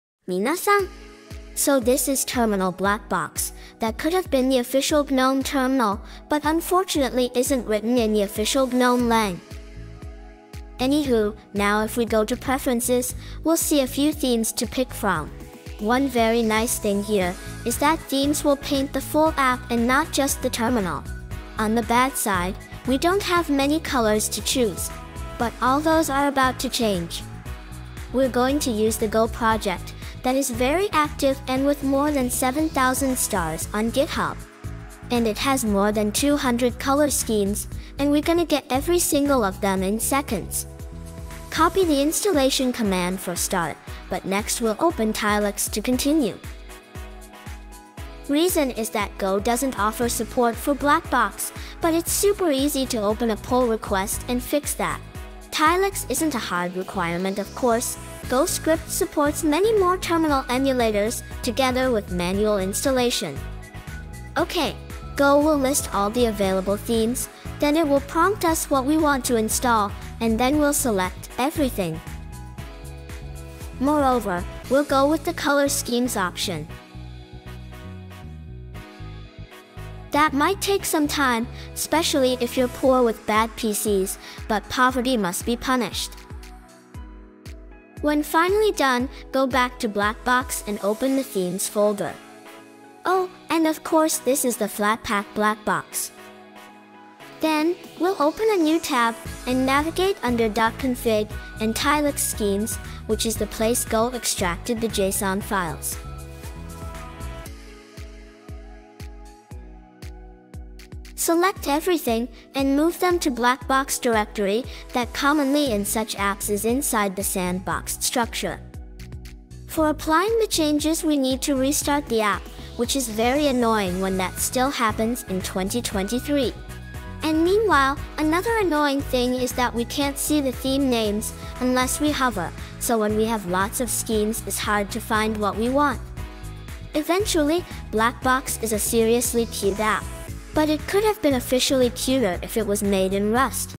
Patching Blackbox with 200+ themes from Gogh | Ft GNOME 43
Gogh is a super project offering color schemes for your terminal!!

Background music is ai generated from Soundraw